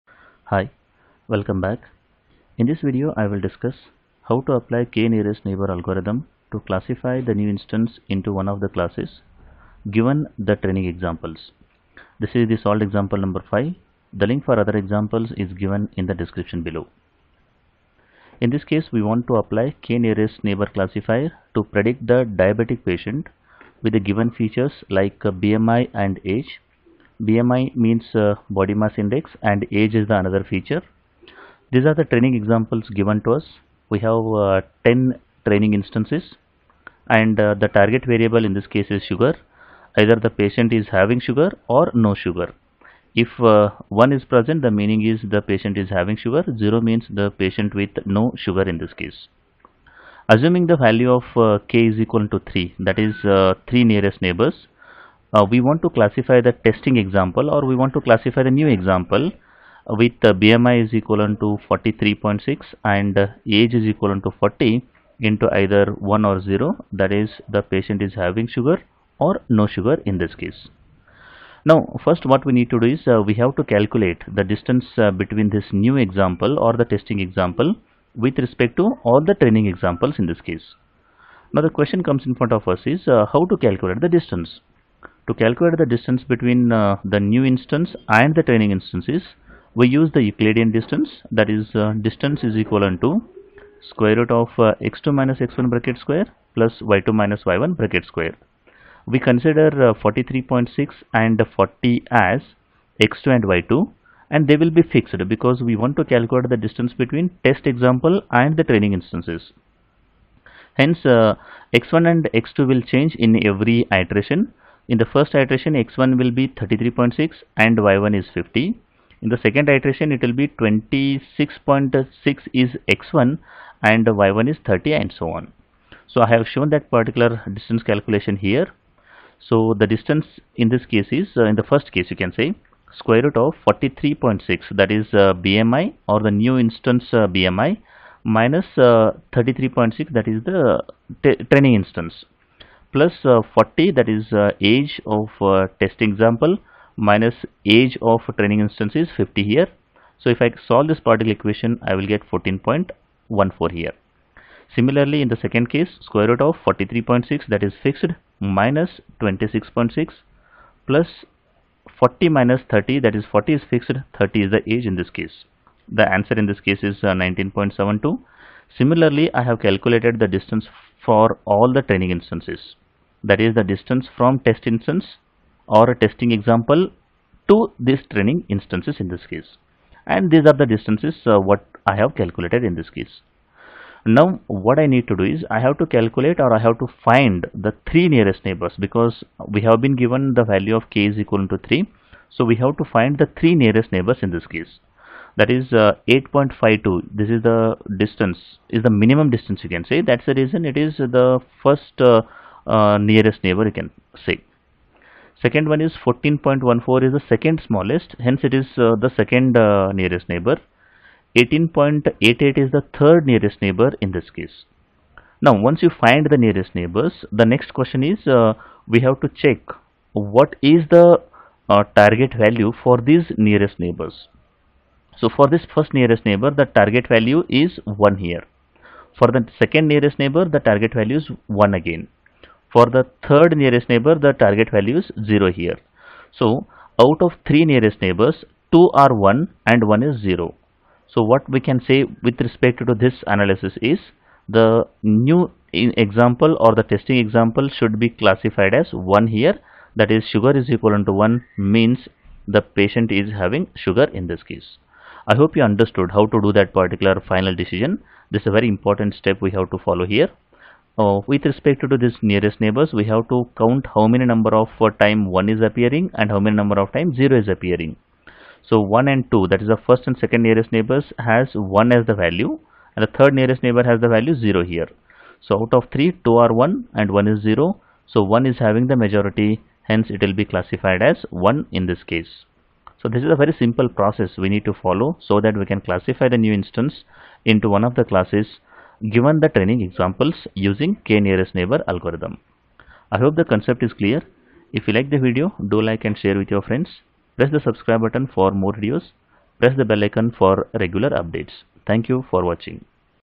K Nearest Neighbors Algorithm to classify Diabetic Patient Sugar given BMI and Age Dr. Mahesh Huddar
Apply K nearest neighbor classifier to predict the diabetic patient with the given features BMI, Age. If the training examples are,
Assume K=3,
Test Example BMI=43.6, Age=40, Sugar=?